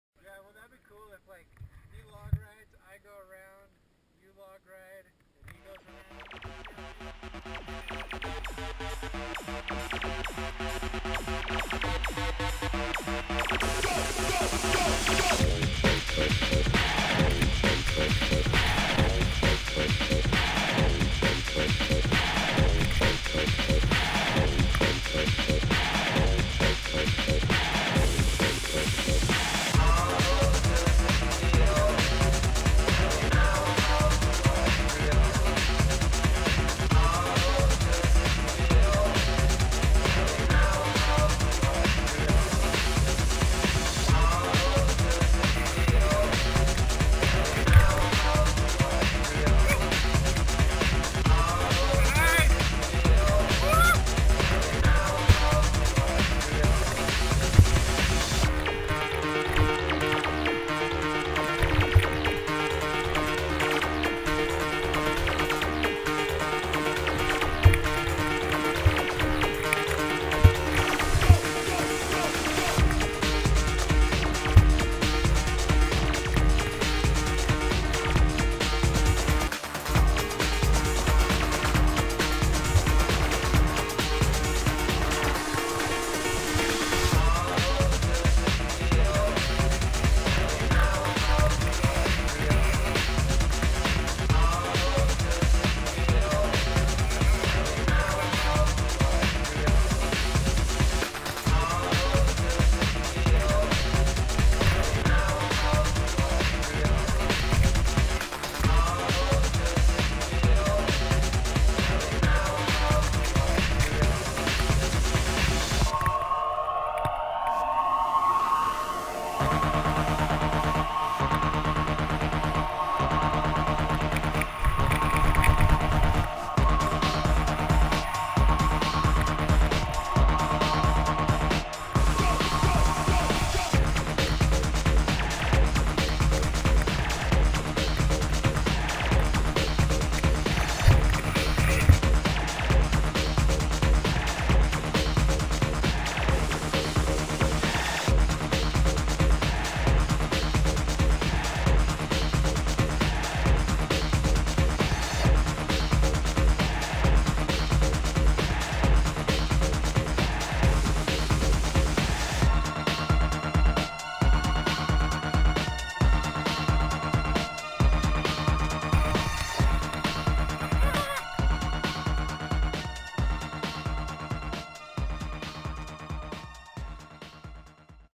Easter Sunday on Bonzai Trail!.wmv
First test ride on the 2012 Norco Aurum. Easter morning 2012 at Black Rock Mountain Bike Area with G, Lou and Luke. No dub step was harmed in the making of this edit.